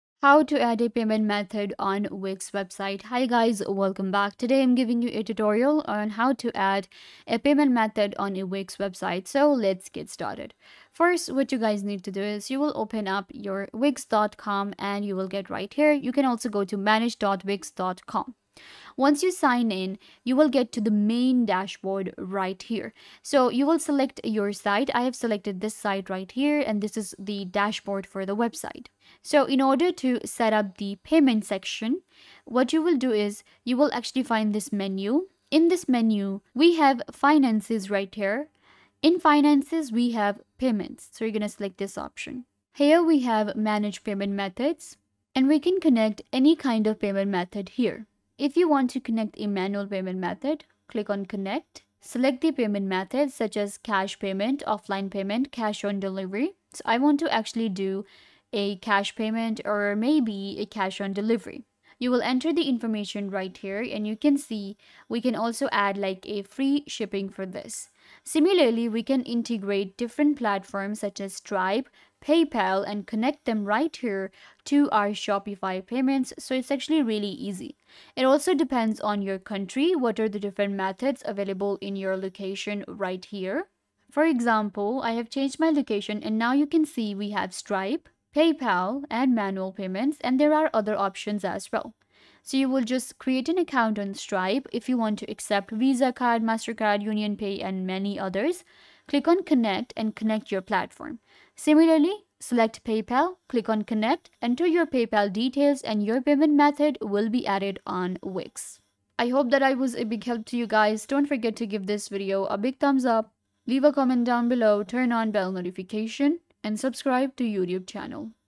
How To Add a Payment Method On Wix Website - Quick And Easy!
Learn How To Add a Payment Method On Wix Website. It is really easy to do and learn to do it in just a few minutes by following this super helpful tutorial to get a better understanding of everything.